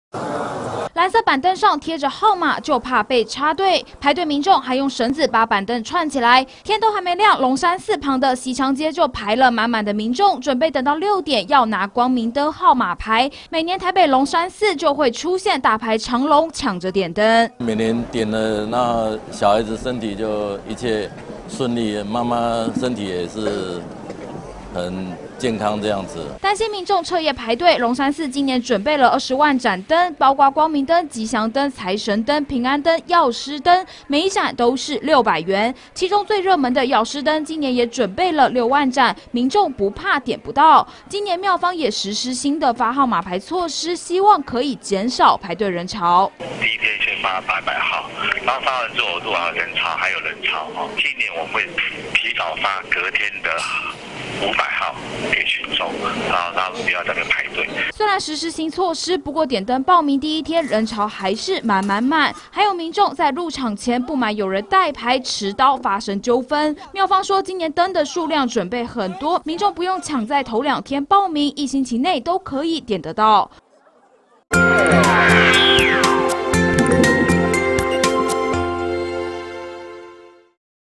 龍山寺點燈報名 信徒半夜拿板凳挑隊【央廣新聞】
距離除夕剩下一個月，台北龍山寺也開始報名點燈，23日早上六點開始發號碼牌，廟方原本希望民眾不要徹夜排隊，今年準備了20萬盞燈，包括最熱門的藥師燈也有6萬盞，光明燈10萬盞，不過還是有很多民眾從半夜就拿著板凳排隊，龍山寺今年採取新措施，原本預計發800張號碼牌，今年第一天就發出1300張，不過還是有民眾不滿有人代排，廟方也說，一週內民眾都有機會點得到燈，不用急著在頭兩天徹夜排隊。